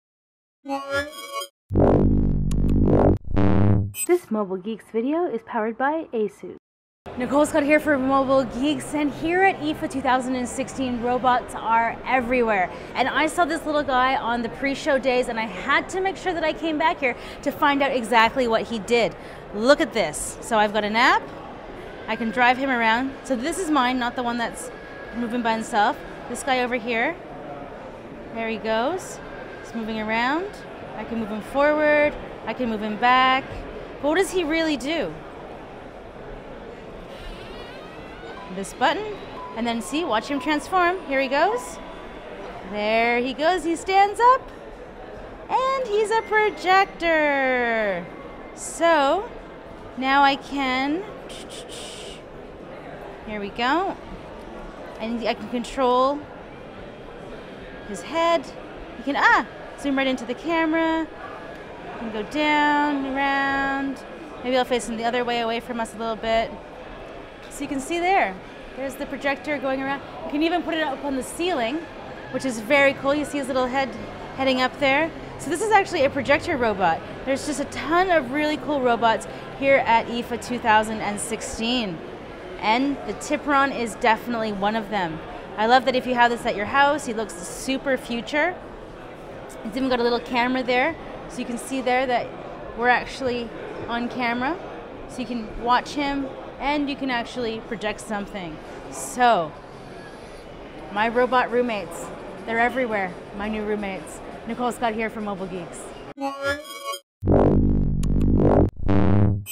Projector robot hands on at IFA 2016
This japanese robot folds up to full size with a push of a button, uses its wheels to drive around and throws an up to 80 inch large image with its built in projector.